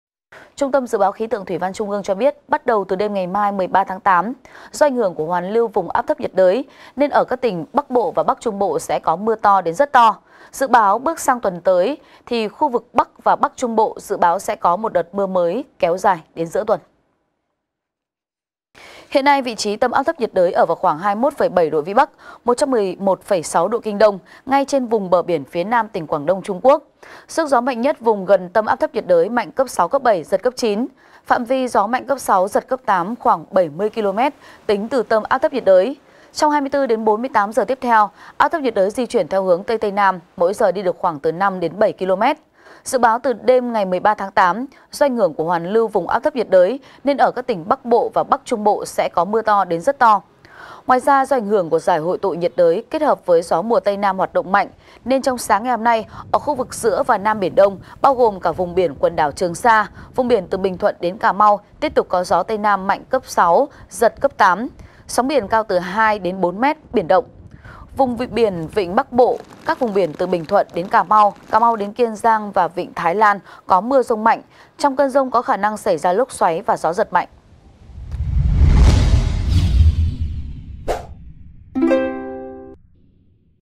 Áp thấp nhiệt đới gây mưa to đến rất to ở Bắc và Bắc Trung bộ | VTC14
VTC14 | ÁP THẤP NHIỆT ĐỚI GÂY MƯA TO ĐẾN RẤT TO Ở BẮC VÀ BẮC TRUNG BỘ
Trung tâm Dự báo Khí tượng Thủy văn Trung ương cho biết, bắt đầu từ đêm ngày mai 13/8 do ảnh hưởng của hoàn lưu vùng áp thấp nhiệt đới nên ở các tỉnh Bắc Bộ và Bắc Trung Bộ sẽ có mưa to đến rất to.
Dự báo bước sang tuần mới, khu vực Bắc và Bắc Trung Bộ dự báo sẽ có một đợt mưa mới kéo dài đến giữa tuần.